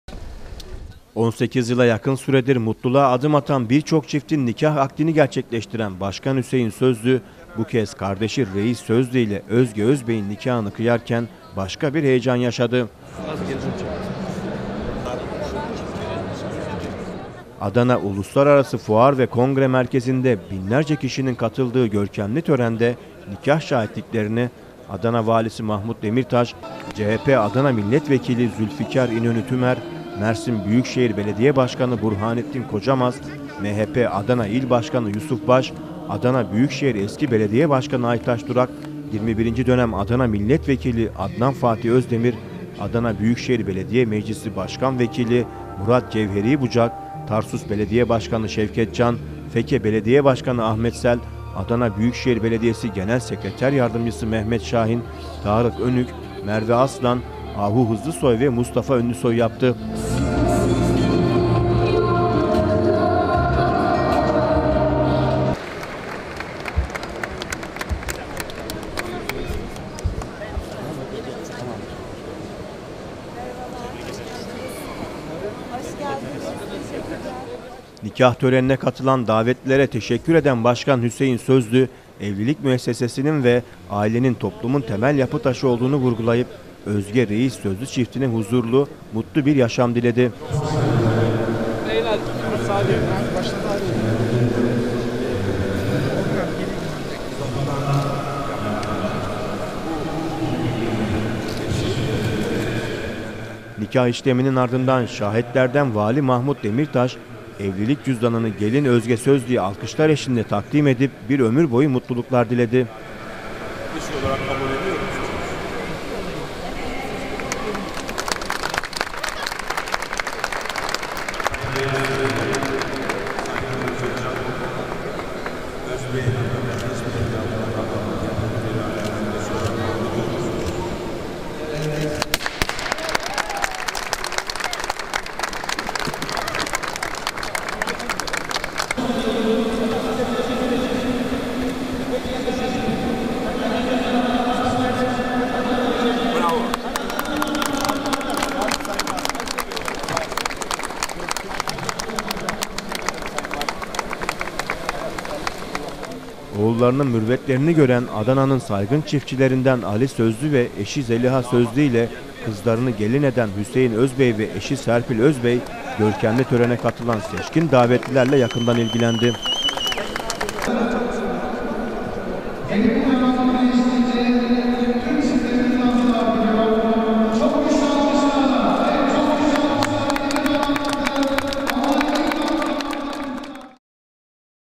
DÜĞÜN ADANA
Adana Büyükşehir Belediye Başkanı Hüseyin Sözlü, dünya evine giren kardeşi Reis Sözlü ile Özge Özbey’in nikahını kıydı. Özge-Reis Sözlü çiftinin mutlu gününe Sözlü ve Özbey ailelerinin yakınları ile Türkiye’nin dört bir yanından siyaset, bürokrasi, iş ve spor dünyası, medya ile sivil toplum örgütlerinin temsilcileri katıldı.